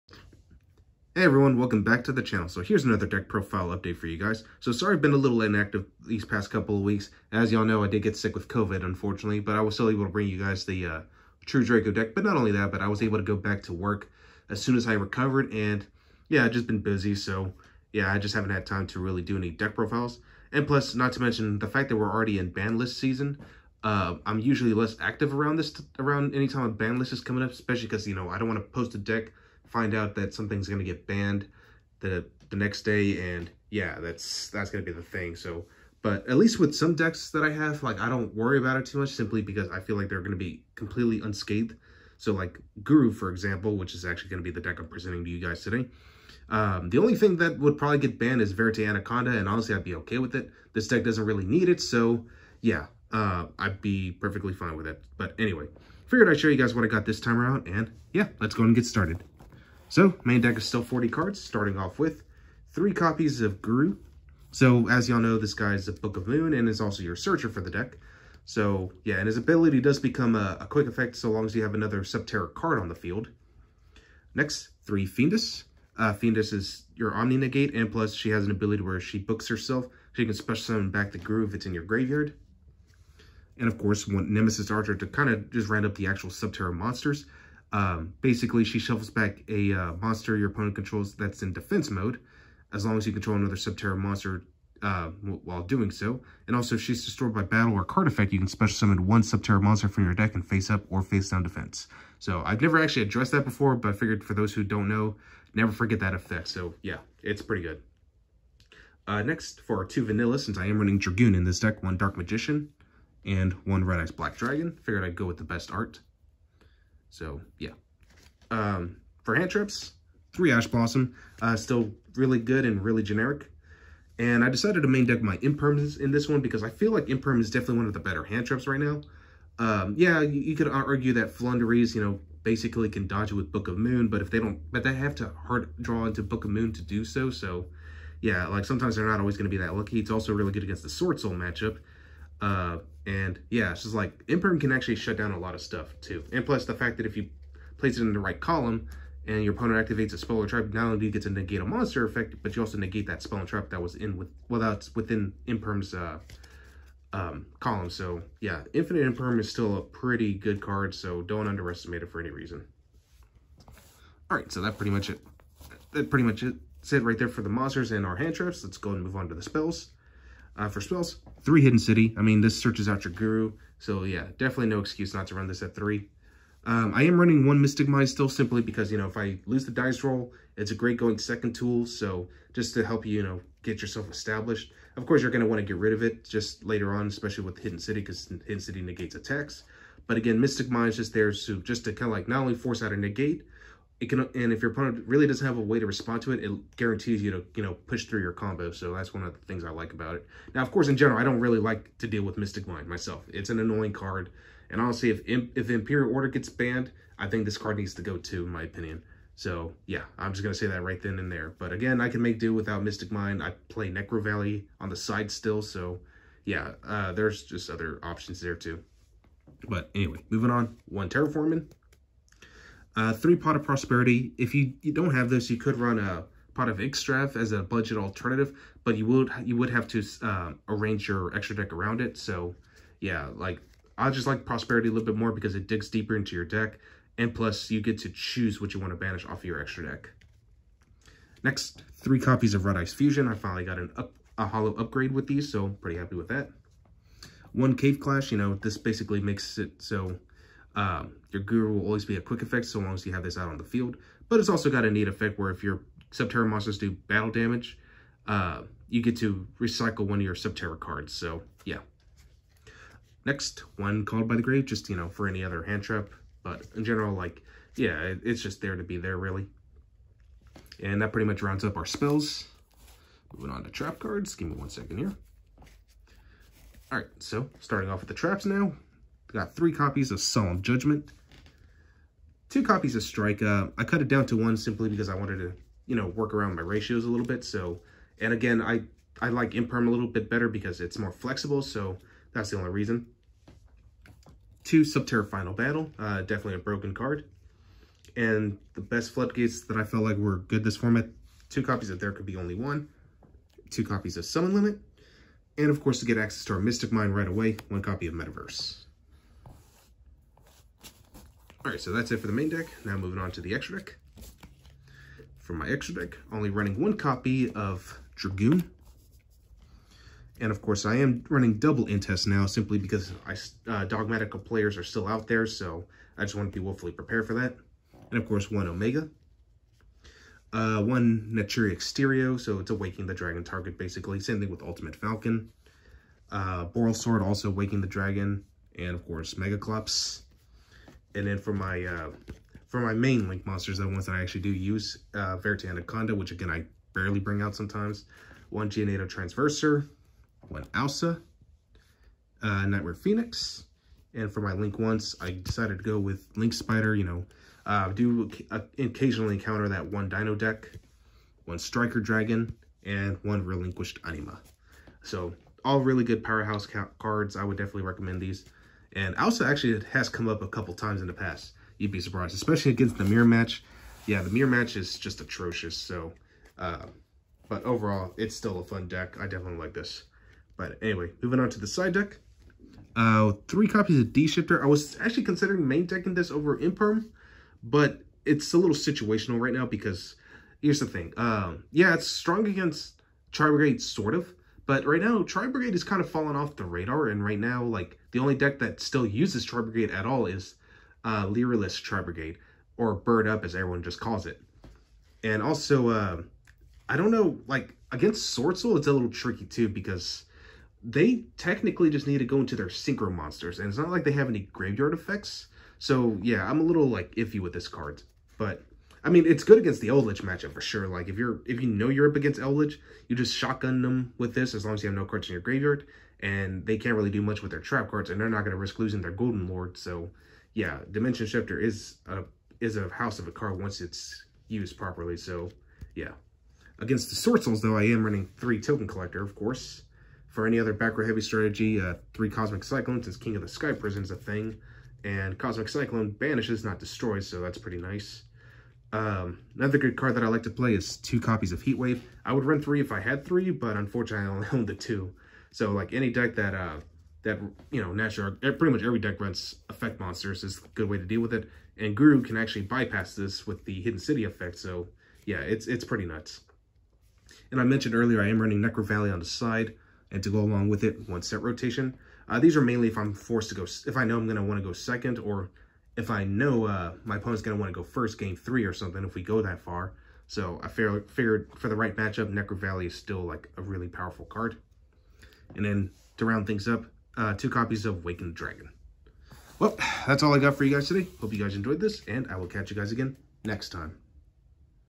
Dragoon Guru Control Deck Profile January 2022
Hey everyone, I meant to bring this out yesterday but I got sidetracked especially because I worked late. Anyway I figured I'd bring you all an updated list on my Dragoon Guru Control Deck. I adjusted my deck with cards I felt were best for this format's match-ups so hopefully this list will be helpful. Please enjoy as always!